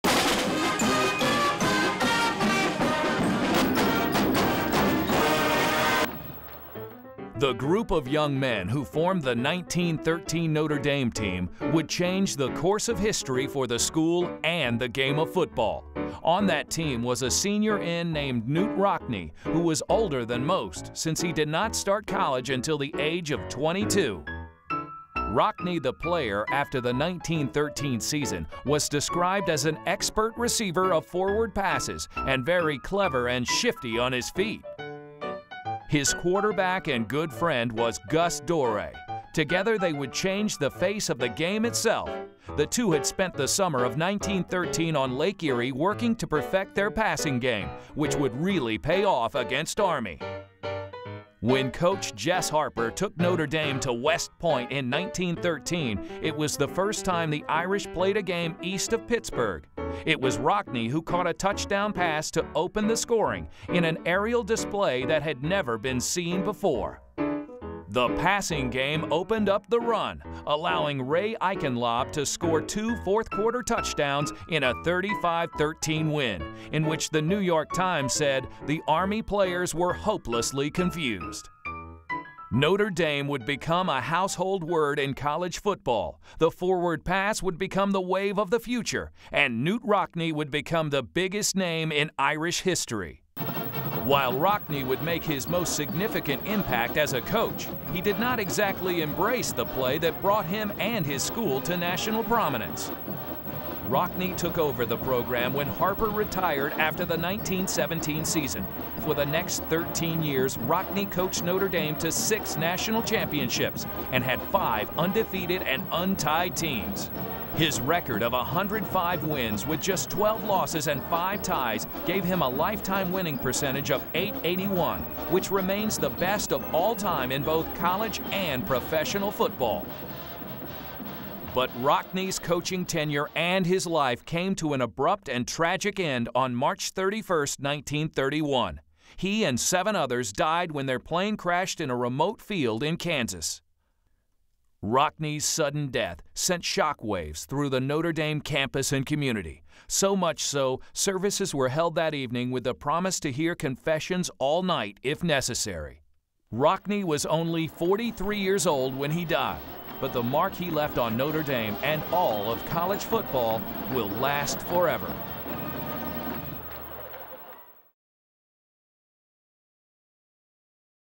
#14 1913: Notre Dame 35 Army 13 - Notre Dame Golden Moments
Notre Dame Golden Moments